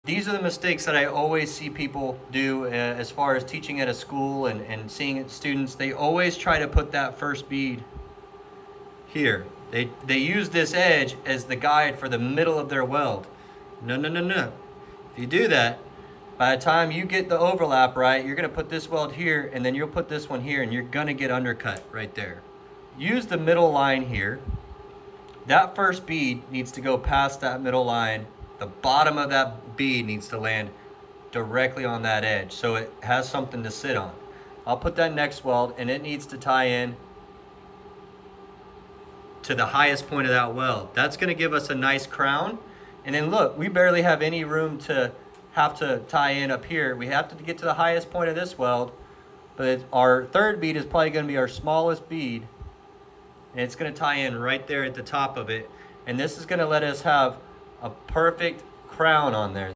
Proper Bead Placement
Bead placement is key when welding out a groove weld and @dr.weldz has a new video over in the weldapp to help you plan out each layer on a 2G open root plate running Tig.